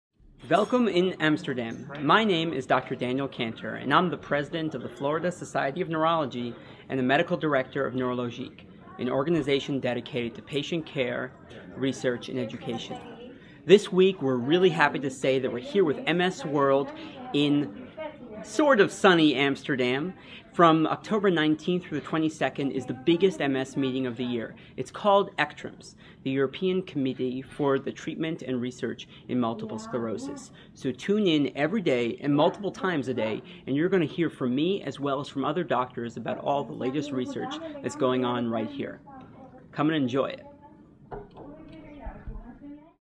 Welcome To ECTRIMS
Dr. Daniel Kantor, President of Florida Society of Neurology and Medical Director of Neurologique, welcome's us all to ECTRIMS on Day 1 of the conference.